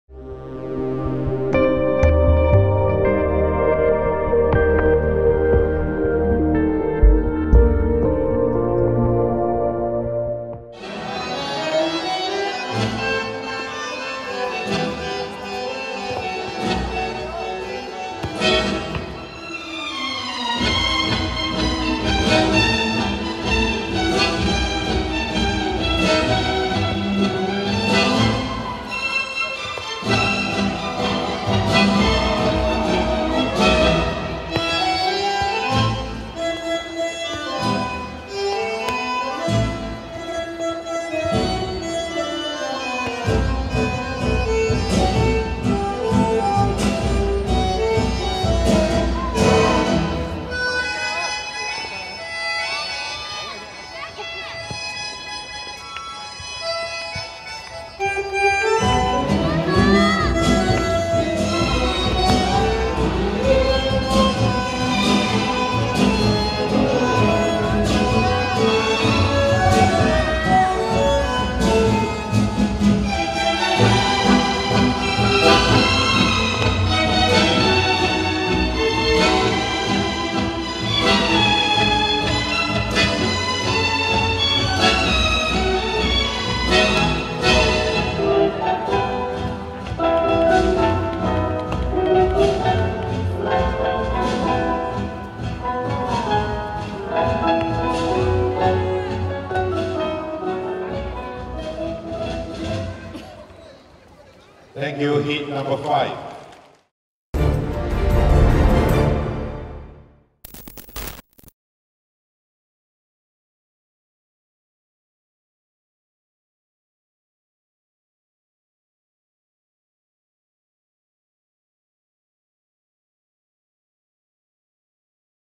Novikov - Klimenko, KAZ | 2014 GS STD Wuhan R1 T | DanceSport Total
Novikov - Klimenko, KAZ | 2014 GS STD Wuhan R1 T

Ivan Novikov - Margarita Klimenko, KAZ, dance the Tango in round 1 of the 2014 WDSF GrandSlam Standard in Wuhan, CHN, on 14 September.